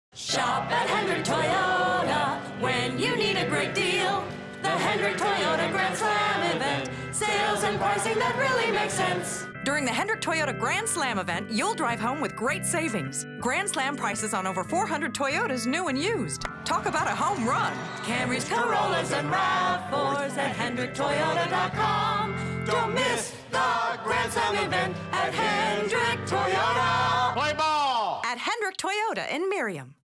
Hendrick Toyota -- KC Royals Grand Slam
This spot in our People Pleasers campaign only ran in the KC Royals TV broadcast.